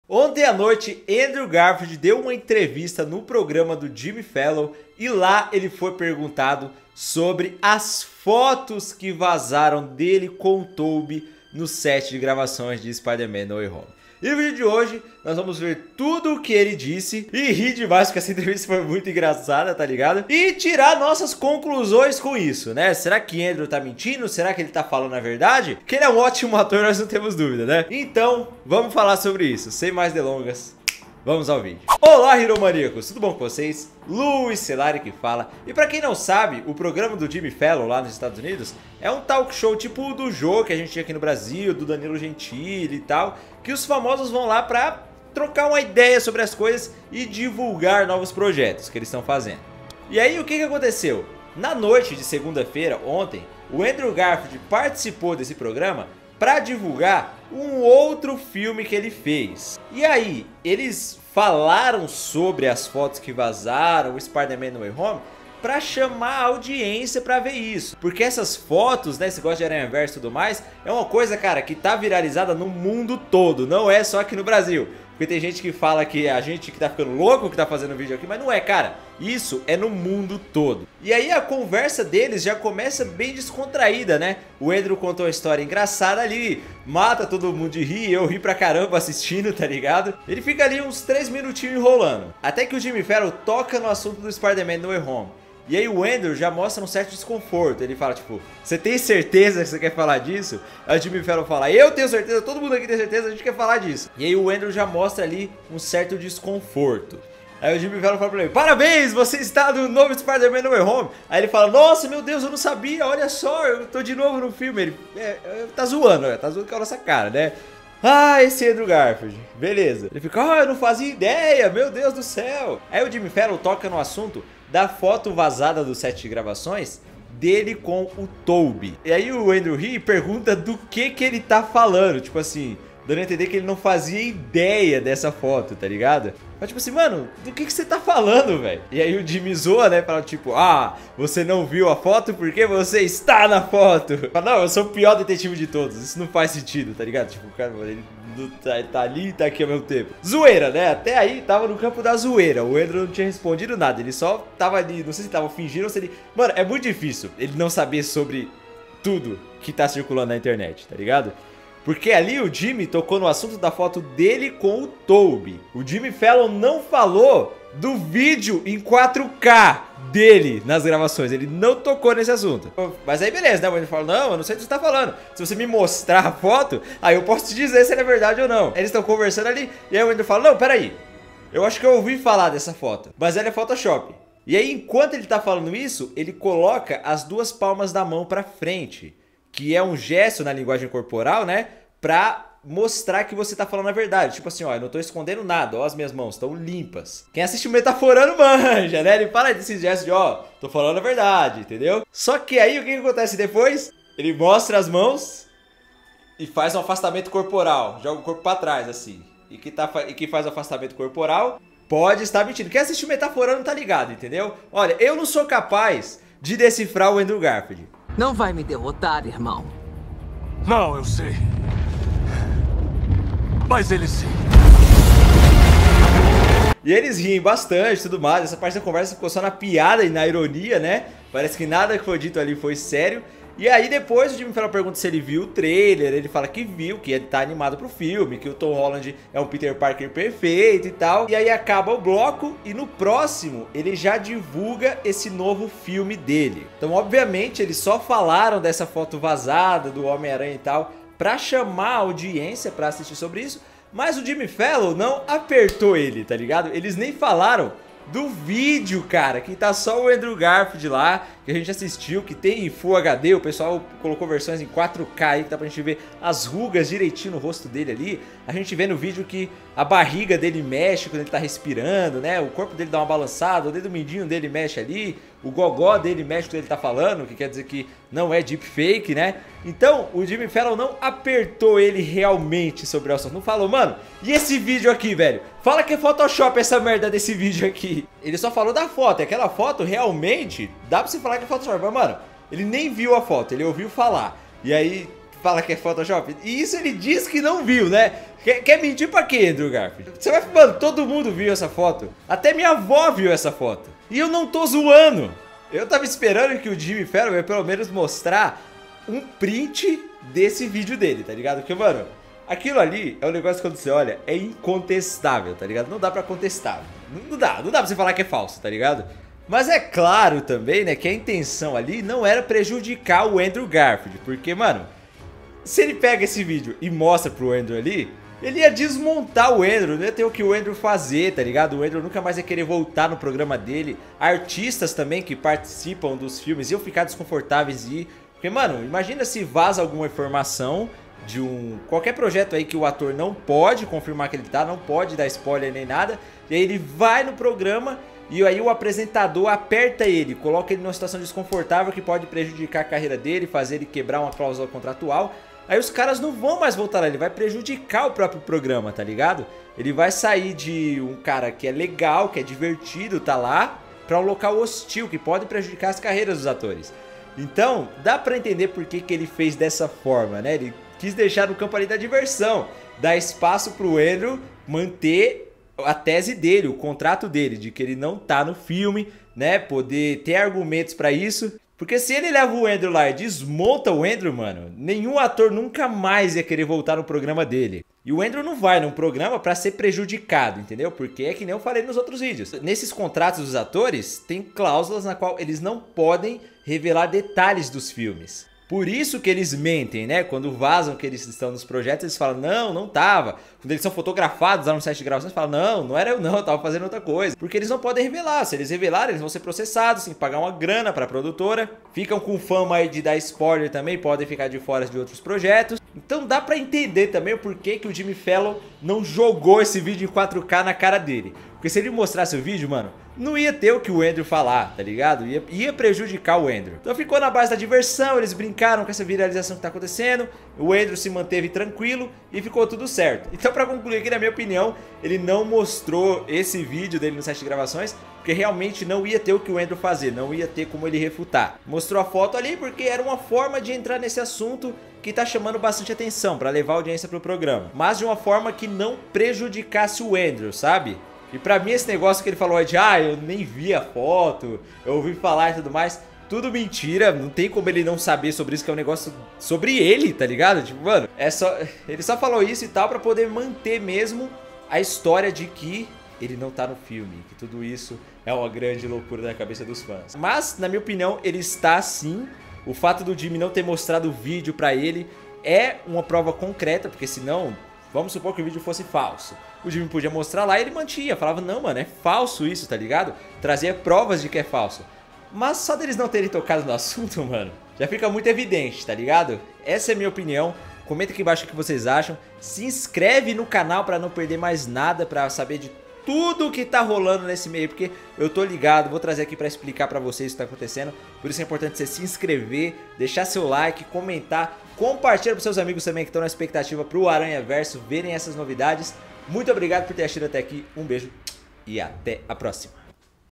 ANDREW RESPONDE CENA COM TOBEY VAZADA DE HOMEM ARANHA 3 NO WAY HOME!!
OLÁ HERO MANÍACOS!! Hoje em:"ANDREW RESPONDE FOTO COM TOBEY VAZADA DE HOMEM ARANHA 3 NO WAY HOME!!" aqui no canal hero manía, vamos falar sobre a entrevista de andrew garfield no jimmy fallon, onde ele fala sobre a foto que supostamente vazou dos sets de gravações de spiderman no way home com tobey maguire e se ele está no novo filme de tom holland no ucm. e ainda diz o que achou do trailer liberado pela sony pictures de homem aranha 3 sem volta para casa

ME SIGA: @luis_sellari